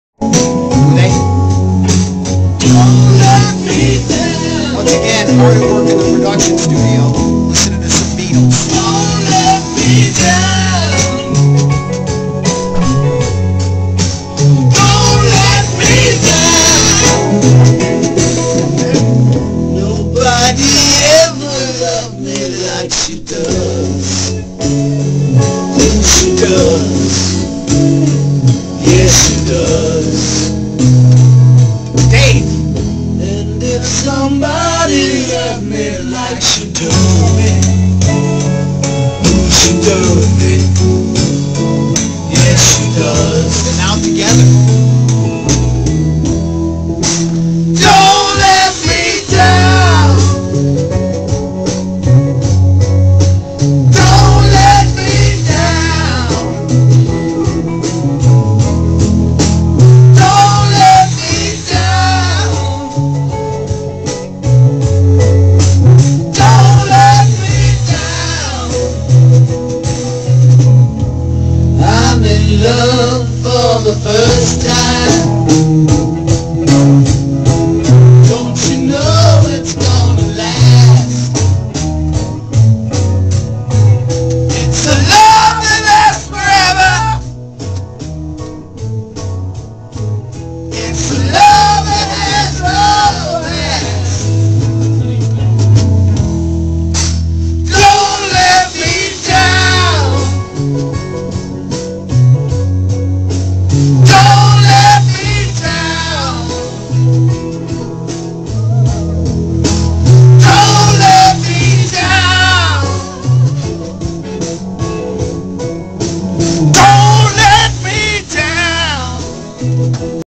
Daily Video Blog: Nick Cellini
Nick loves the Beatles.